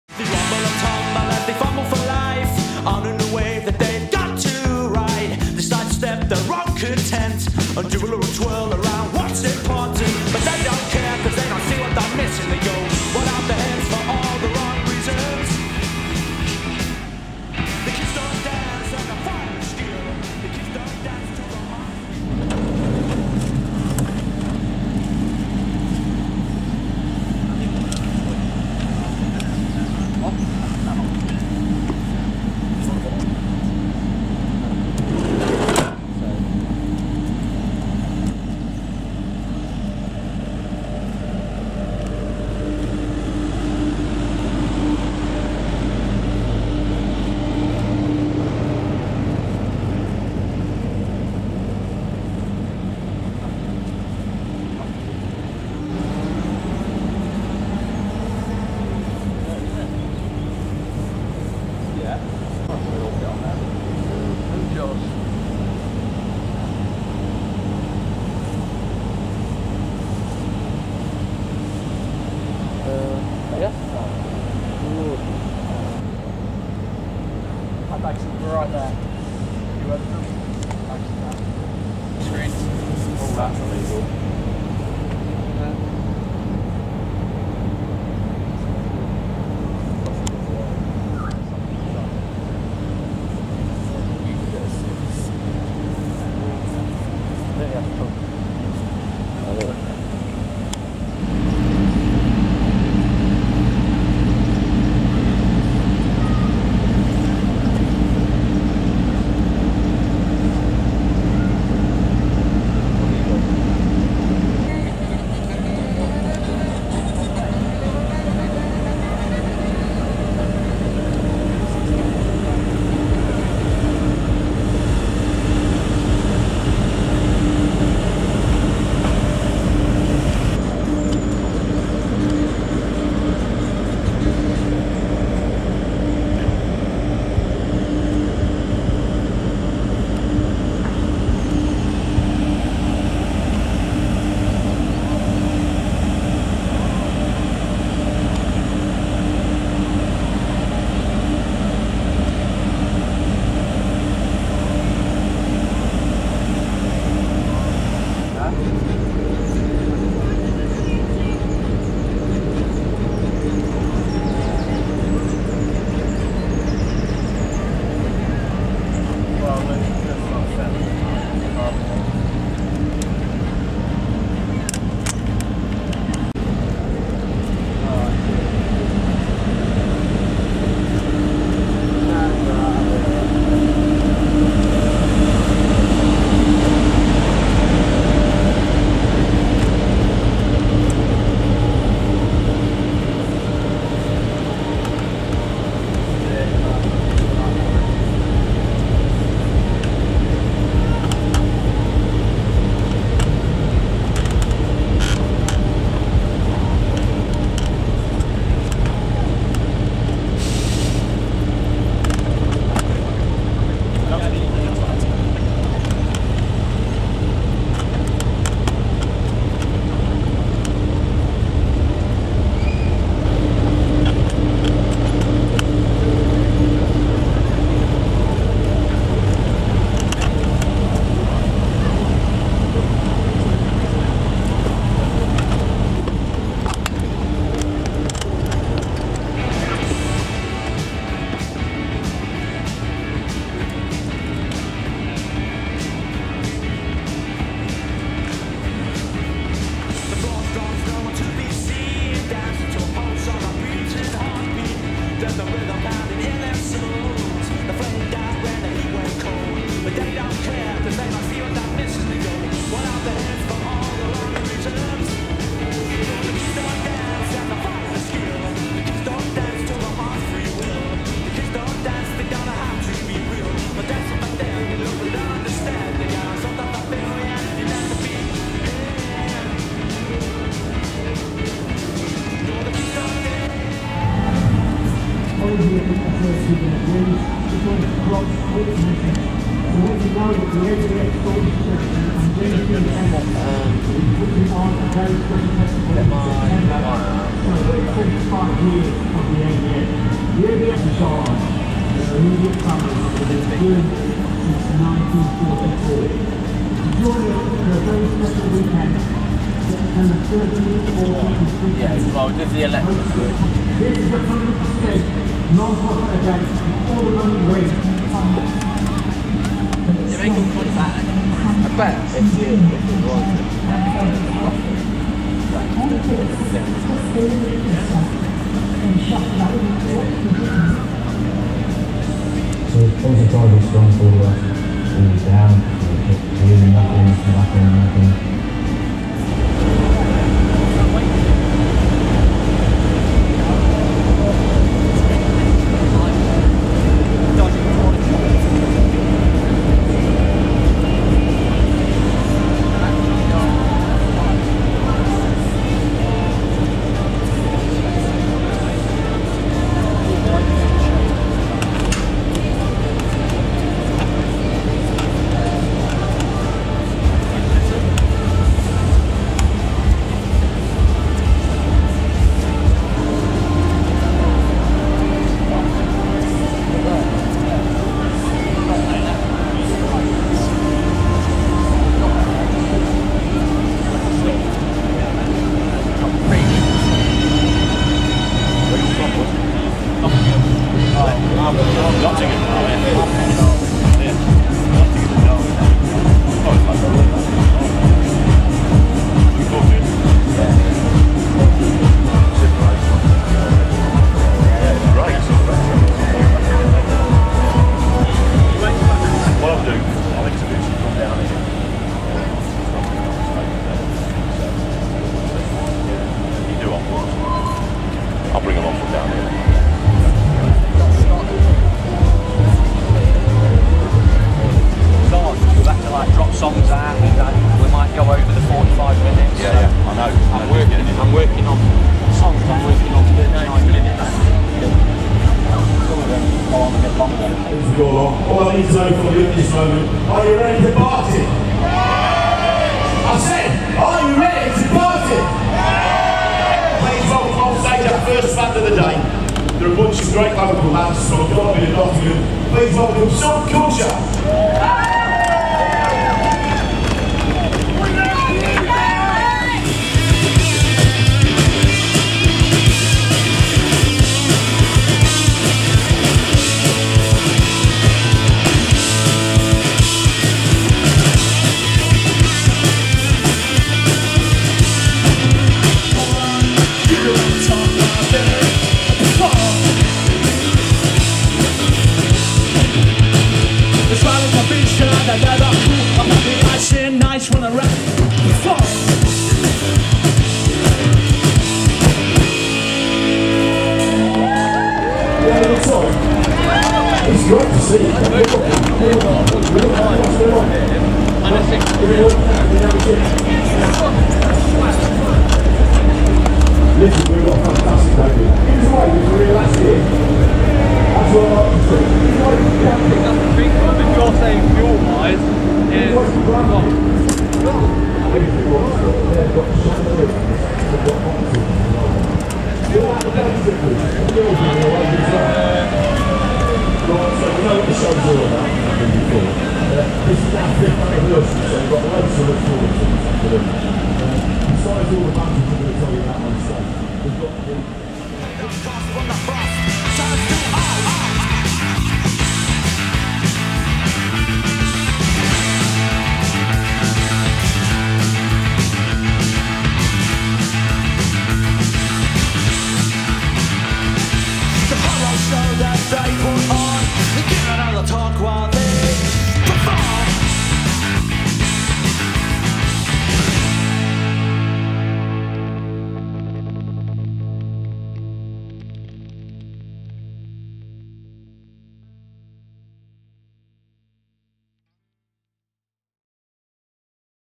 Behind The Scenes Ep.1 | Derbyshire Sausage & Cider Festival
On the 29th June 2019, SubCulture hit Derbyshire Sausage & Cider Festival at Catton Hall. Watch a video diary made up of exclusive behind the scenes footage that was taken from the day.

Get your tickets to our gig at The Chameleon Arts Cafe, Nottingham, on the 18th July: